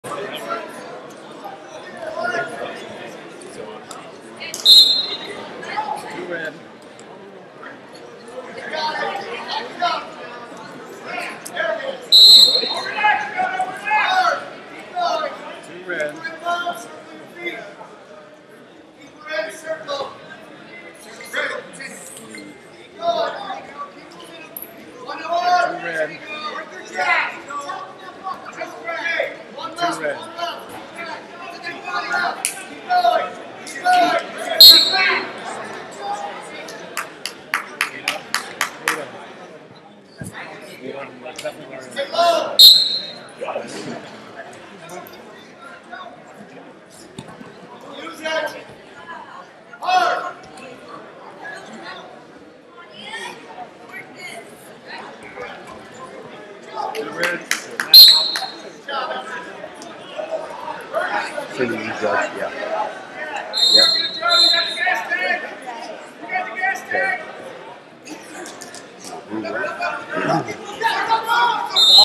50 Kg Prelims Emily Shilson Titan Mercury Wrestling Club Vs Angelina Gomez Lion Wrestling Club
2019 Bill Farrell Memorial International Open - 50 Kg Prelims Emily Shilson Titan Mercury Wrestling Club Vs Angelina Gomez Lion Wrestling Club